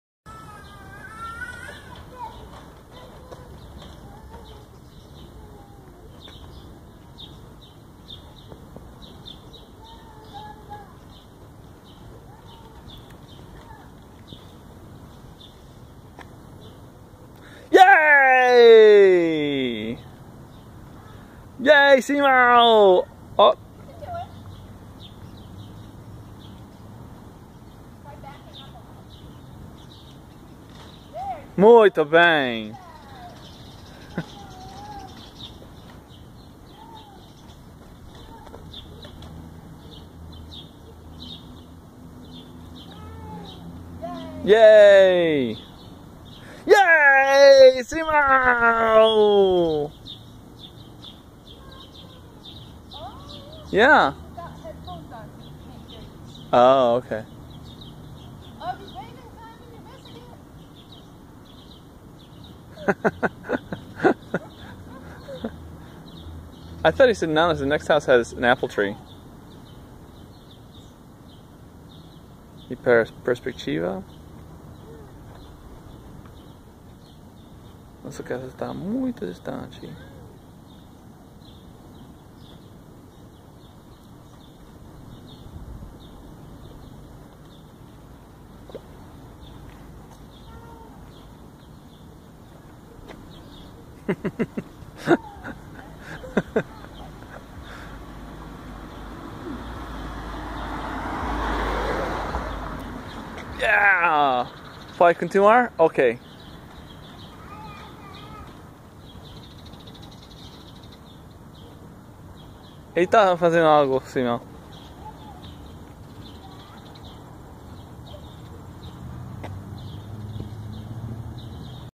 Simon walking up Raleigh St with a cart - shot on poor-quality camera phone
Simon periodically stops to see if his dad is still there and get encouragement. He stops to wave at someone who's sitting on a porch with headphones on.  The cameraman gives a perspective of how far from his house Simon has walked.